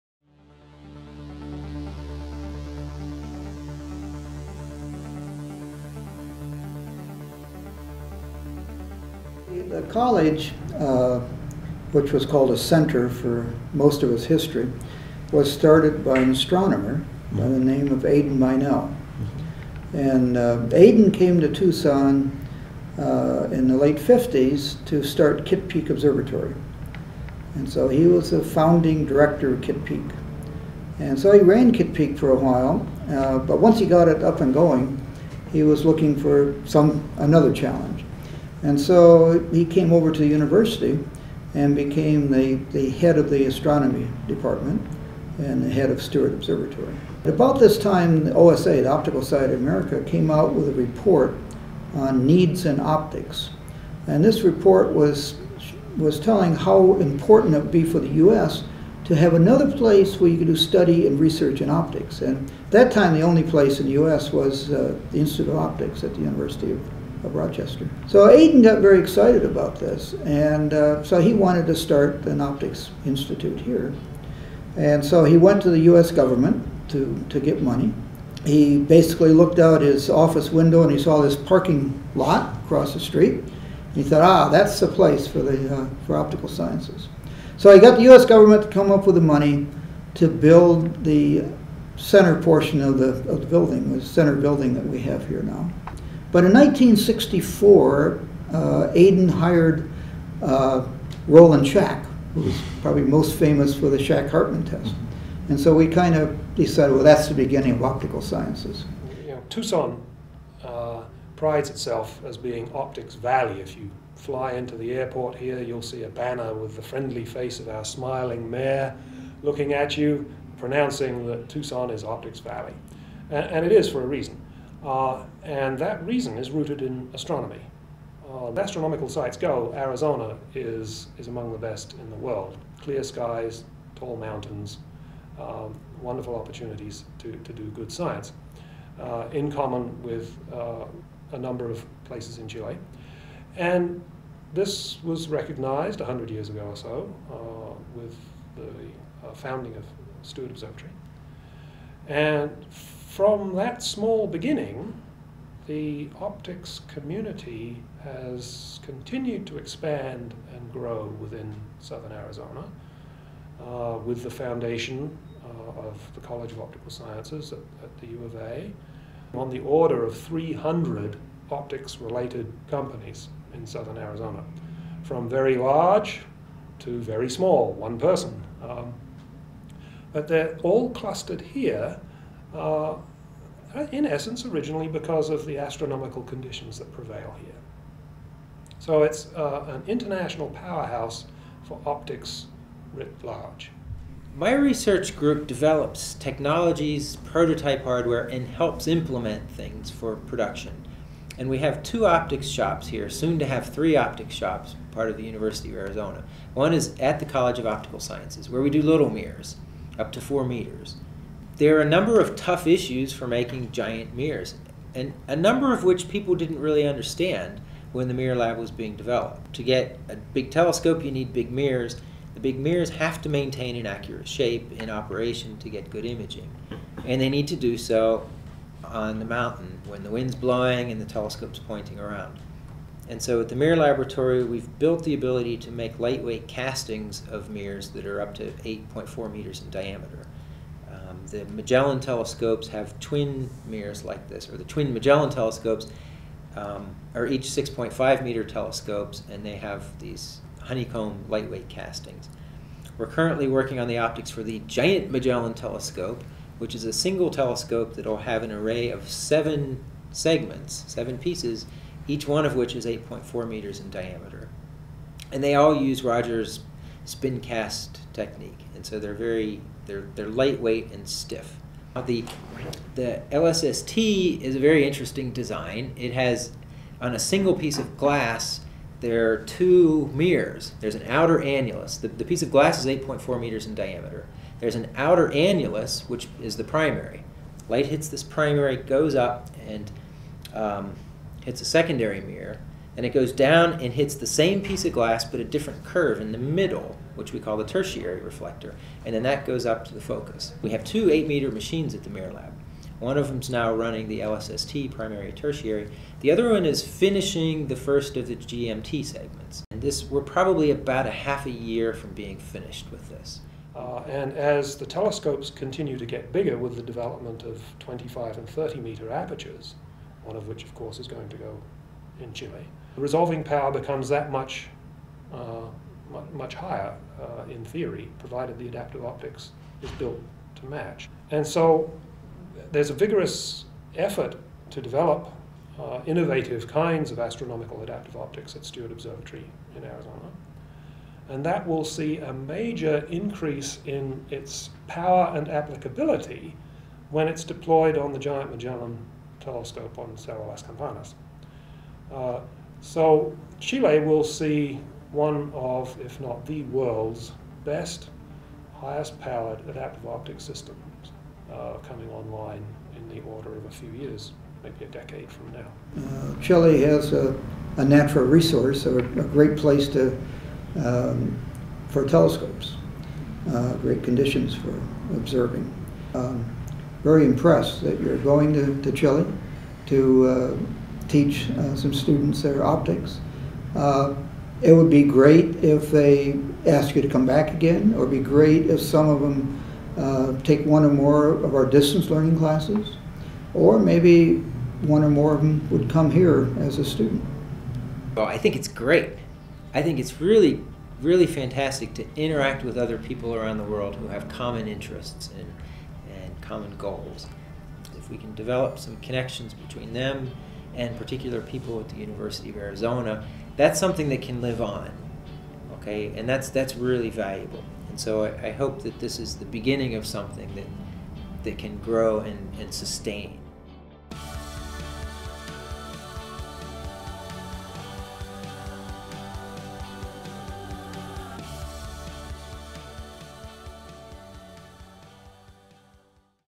Workshop Opening Video
The Astronomical Optics Workshop is an initiative of the Student Optics Chapter and the College of Optical Sciences at the University of Arizona together with the Pontifical Catholic University of Chile. The 4-day workshop focuses on geometrical optics, aberrations, diffraction, and opto-mechanics, emphasizing their applications for telescopes, instrumentation, and any other opto-mechanical system.

Additionally, the program features outreach to several high schools in Santiago with the goal of motivating young students in the sciences and optics and inspiring their participation in the future of astronomy and optics in Chile.